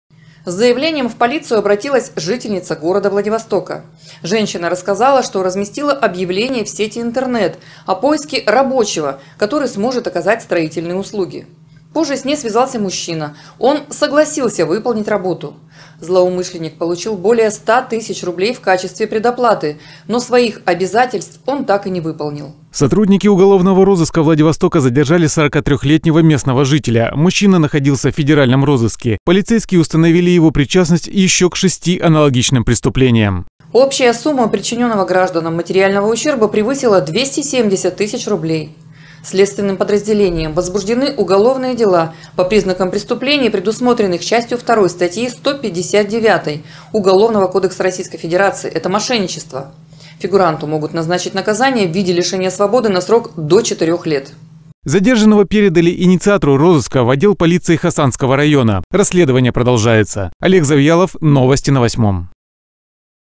Во Владивостоке полицейские задержали мужчину, находящегося в федеральном розыске.
Подозреваемый брал деньги, но обещанные строительные работы не
выполнял. Общий ущерб по 7 установленным эпизодам его противоправной деятельности превысил 270 тысяч рублей.